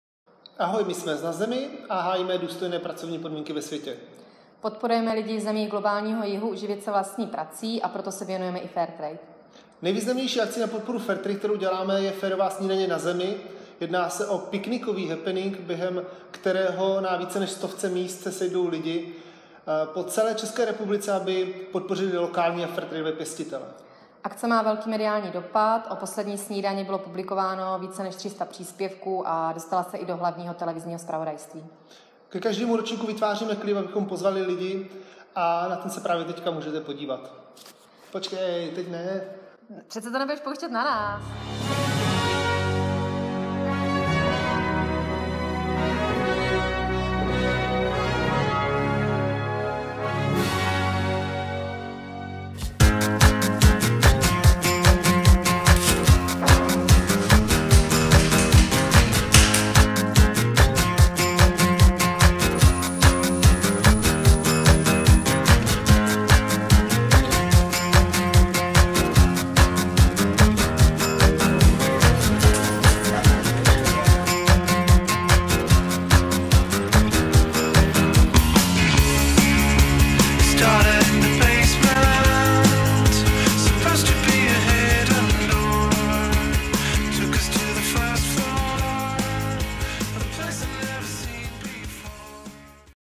Love your producer EYD NaZemi
This short movie is describing the action "Fair Breakfast" that our team from NaZemi nationwide coordinates. Actually it's a picnic happening to support local and fair trade producers. We are pleased for more than 100 organizators in Czech cities taking part.

This short movie competes to participate in this year's Shining Stars of Europe video contest on Development Cooperation.